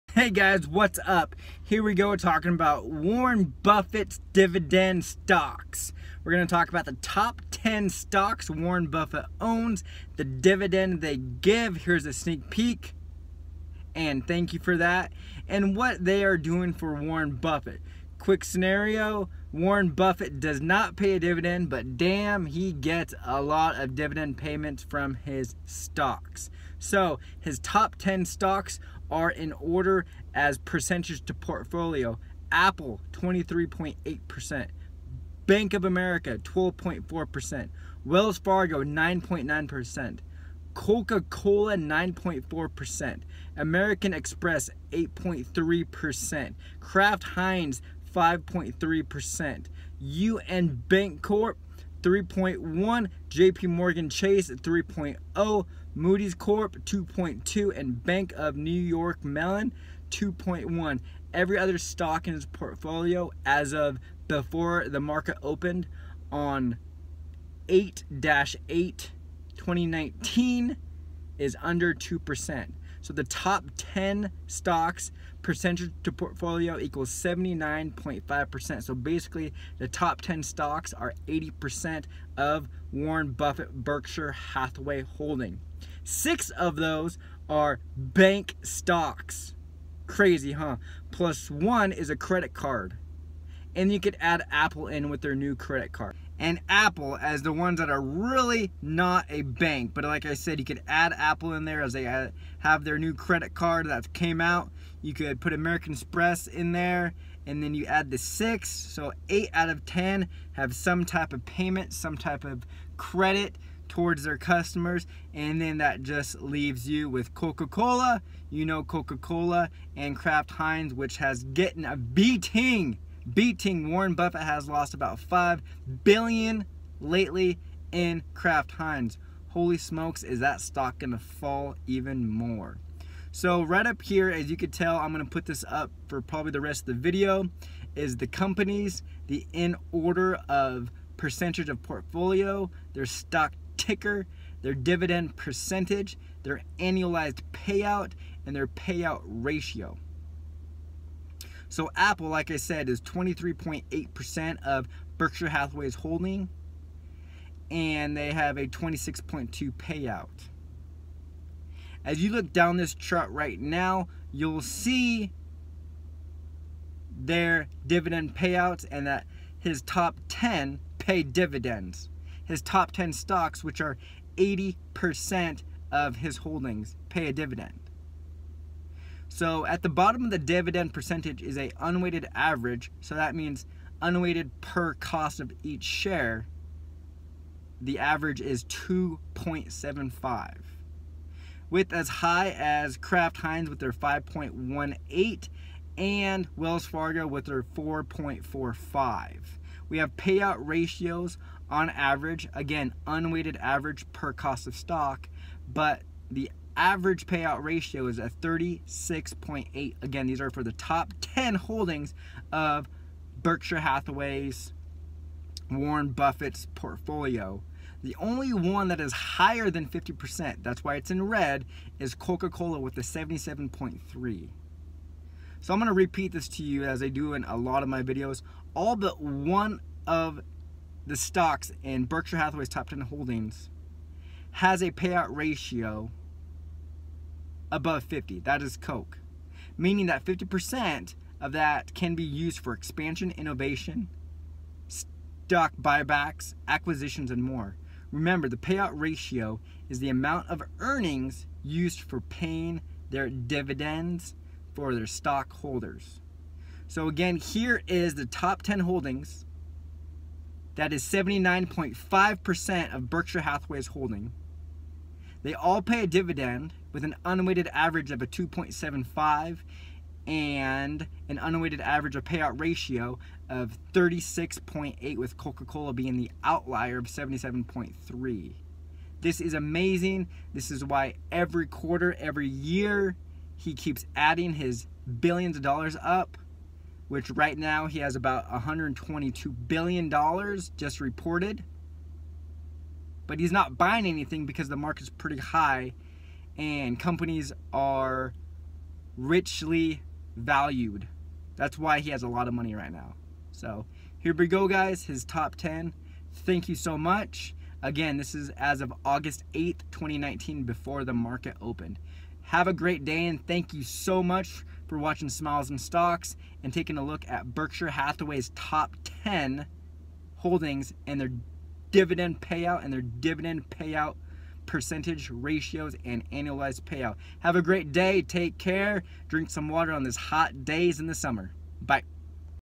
Warren Buffett Dividends : Berkshire Hathaway Dividend Portfolio
AS OF 8/8/2019 BEFORE MARKET OPENED
WARREN BUFFETT TOP TEN HOLDINGS
1) Apple
2) Bank of America
3) Wells Fargo
4) Coca Cola
5) American Express
6) Kraft Heinz
7) United Bancorp
8) JP Morgan Chase
9) Moody's
10) Bank of New York Mellon